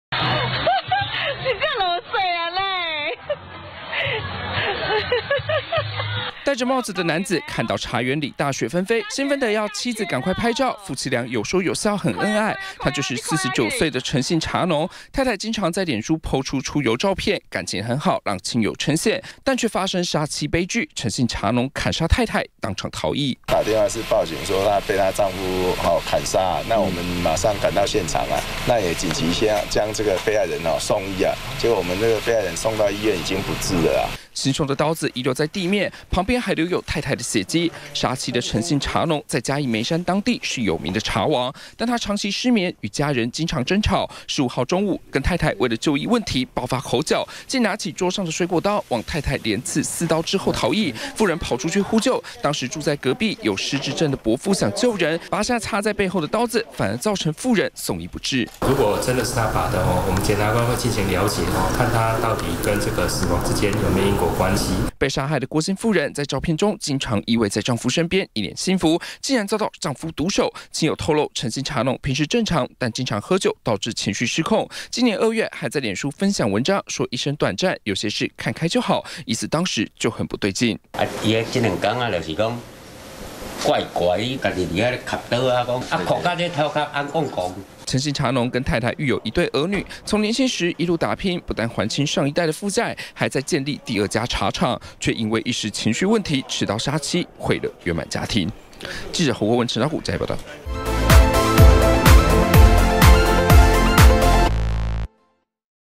20161216中天新聞 失眠3天情緒失控 「茶王」雙刀殺死妻
嘉義縣梅山鄉，15號發生，一名陳姓製茶大王，持刀殺死妻子逃逸，警方調查發現，被砍的太太受傷後，一度還能向外呼救，但當時住在隔壁，有失智症的伯父，看到男子太太傷重，拔下插在背後的刀子，反而造成婦人送醫不治，目前檢方正在釐清相關刑責，但親友透露，這對夫妻其實感情很好，還經常在臉書po出一同出遊的照片，沒想到發生悲劇。